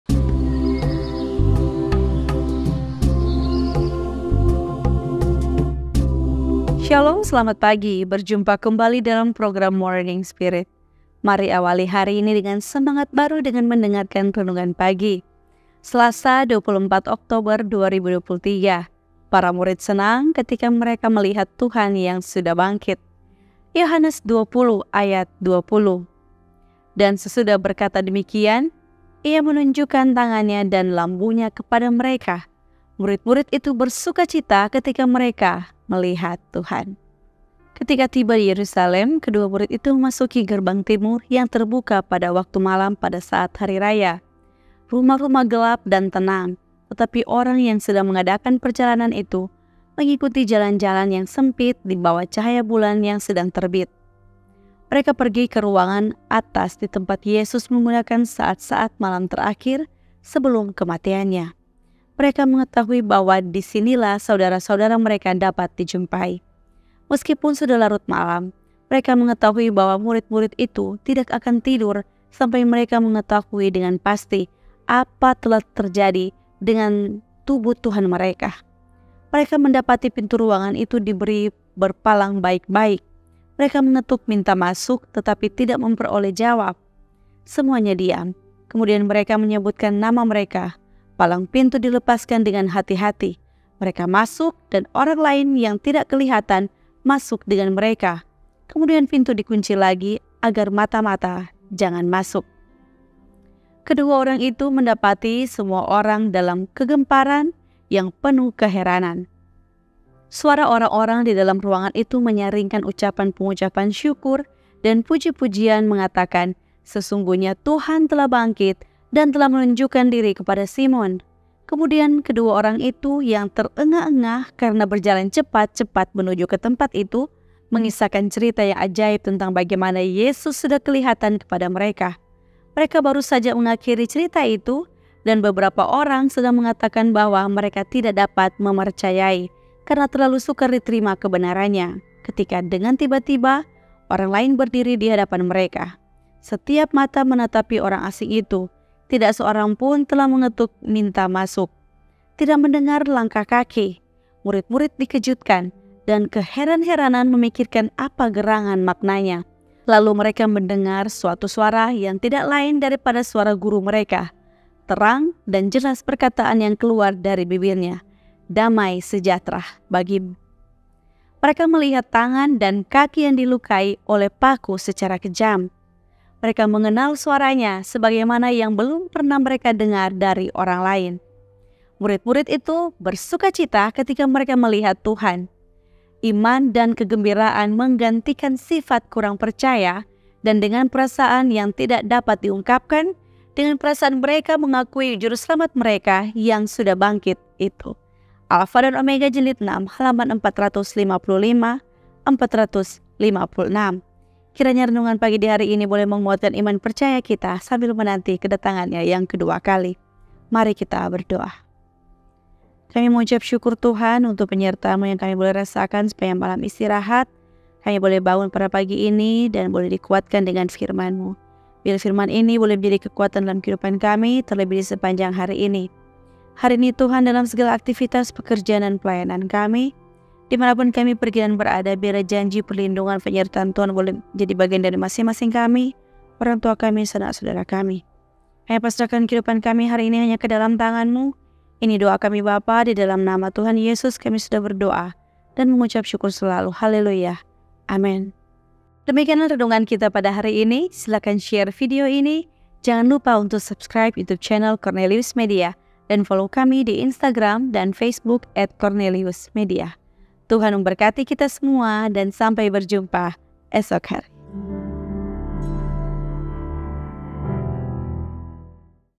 PARA MURID SENANG KETIKA MEREKA MELIHAT TUHAN YANG SUDAH BANGKIT | RENUNGAN PAGI HARI INI
►▷► MORNING SPIRIT: RENUNGAN PAGI KRISTEN HARI INI
PARA MURID SENANG KETIKA MEREKA MELIHAT TUHAN YANG SUDAH BANGKIT
“Dan sesudah berkata demikian, Ia menunjukkan tangan-Nya dan lambung-Nya kepada mereka. Murid-murid itu bersukacita ketika mereka melihat Tuhan” (Yohanes 20: 20).